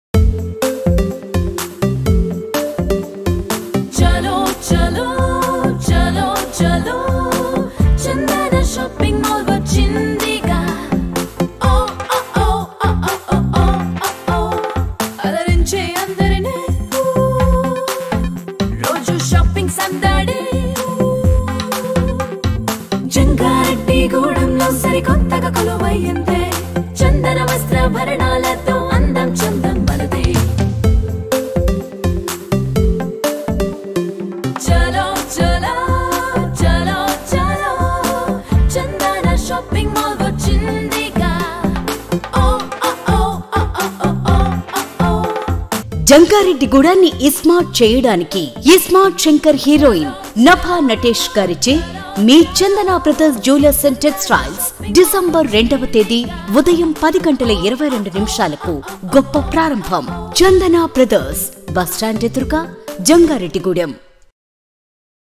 Chandana brother's show room coming to jangareddygudem
Its opening on December 2nd 2019